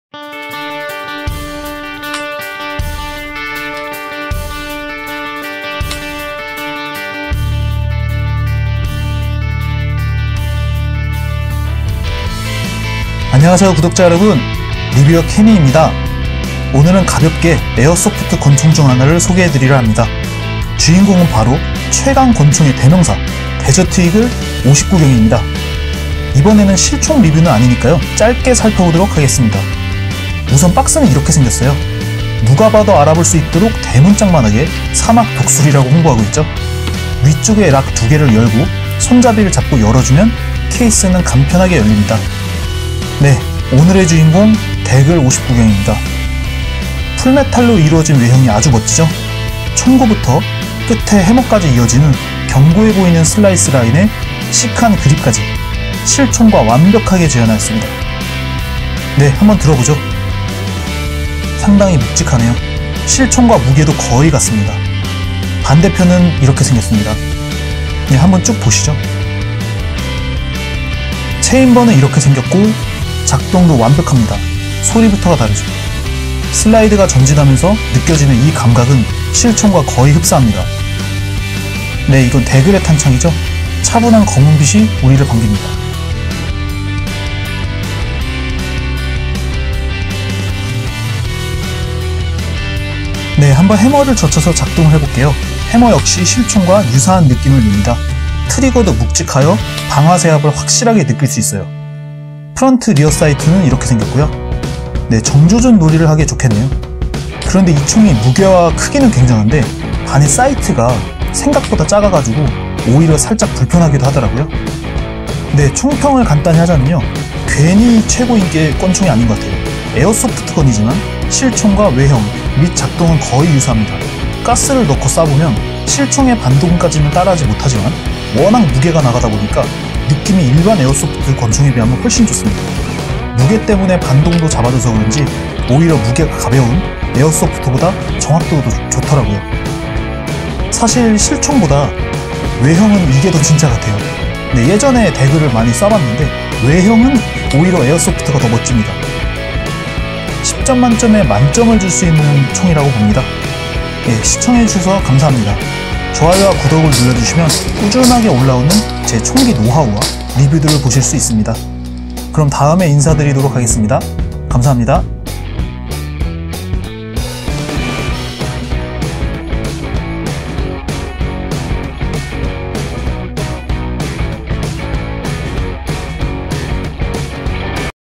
최강 권총의 대명사? 에어소프트 권총: 데저트이글 0.50 구경 리뷰! - Desert Eagle 0.50 -201023
최강 권총의 대명사! 데저트이글 0.50 에어소프트건 리뷰입니다!
실총과 싱크로율 100%!!!  무게감과, 작동도 완벽!

실제 총보다 더 진짜같은 신기한(?) 에어소프트건이 있다?

제 영상을 선택해주셔서 감사합니다 ^^
감사합니다.